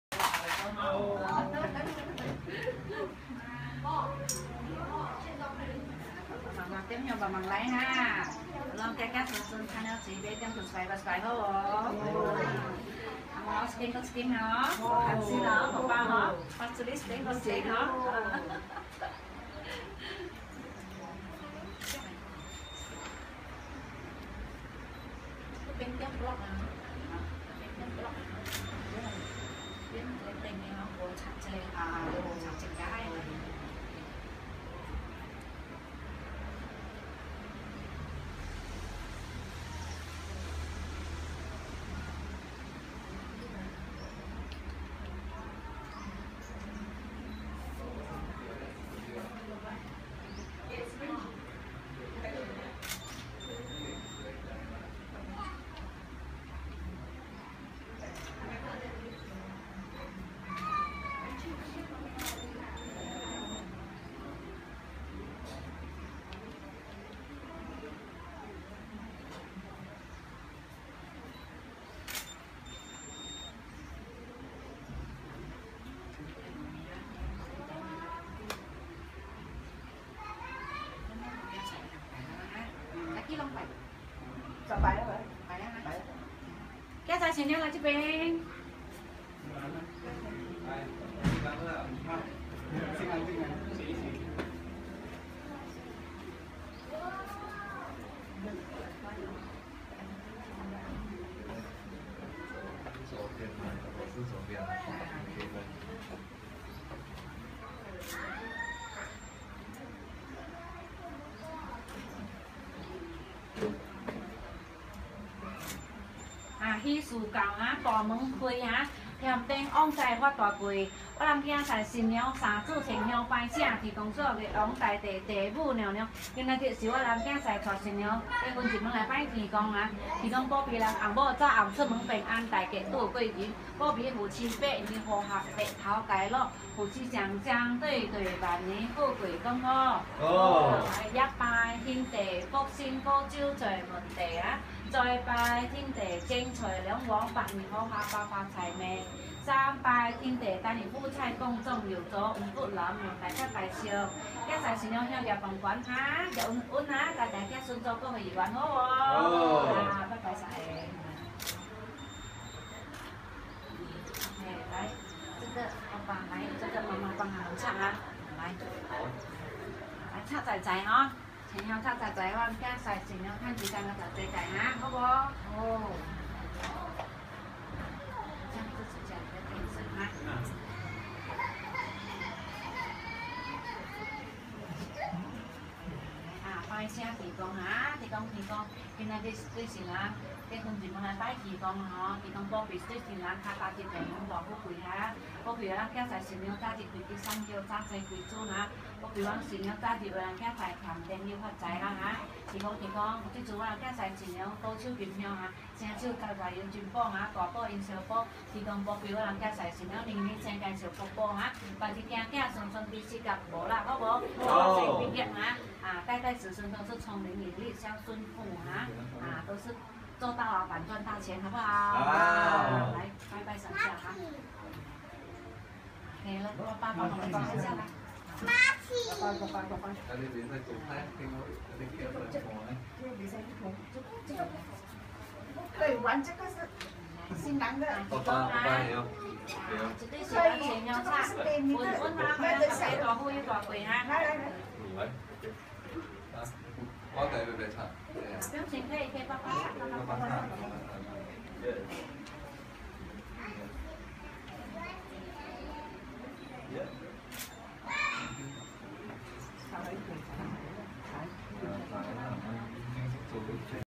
Tea Ceremony Day--ChuanYuin & DeePing (1 of 8)
Chinese Wedding Tradition: Tea Ceremony on 21 December, 2014.